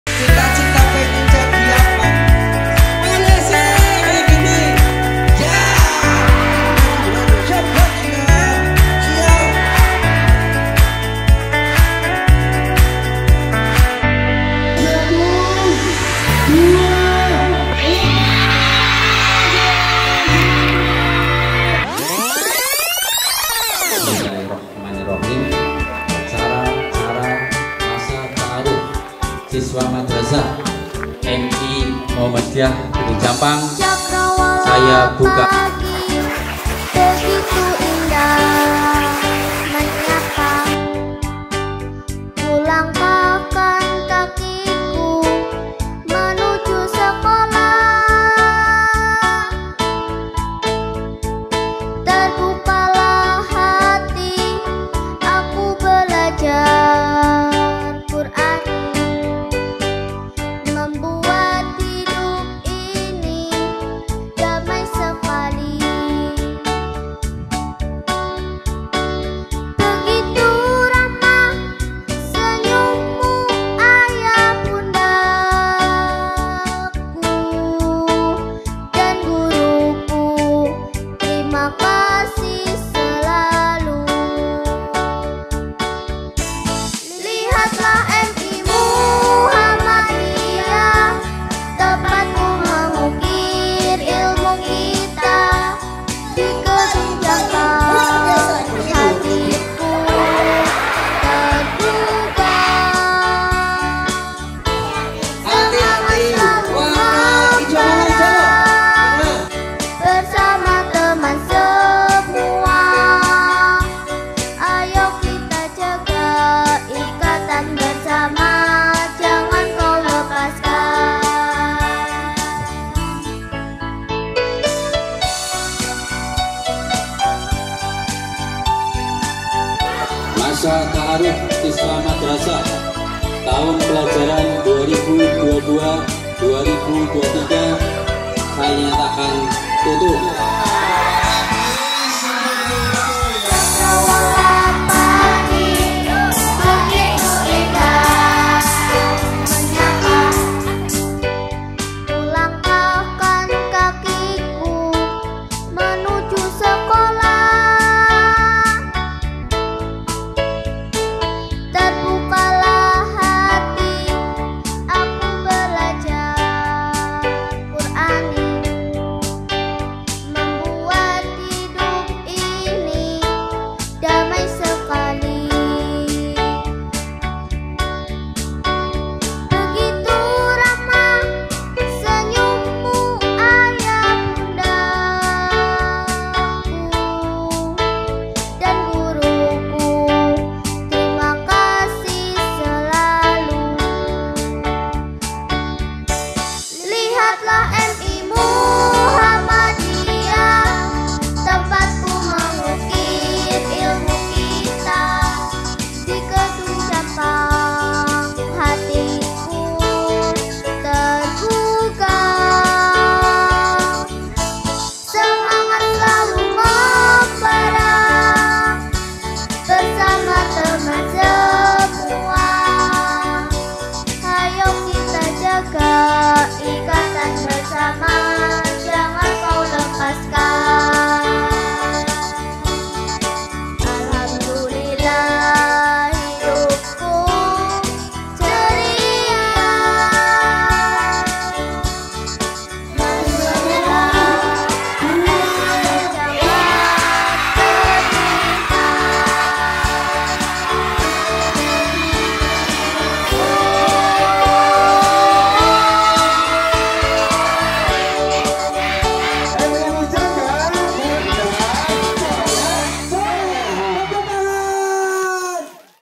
MATSAMA MI MUJANGGA |Masa Ta'aruf Siswa Madrasah Tahun 2022
Alhamdulillah. Masa Ta'aruf Siswa Madrasah awal Tahun Pelajaran 2022/2023 berjalan dengan baik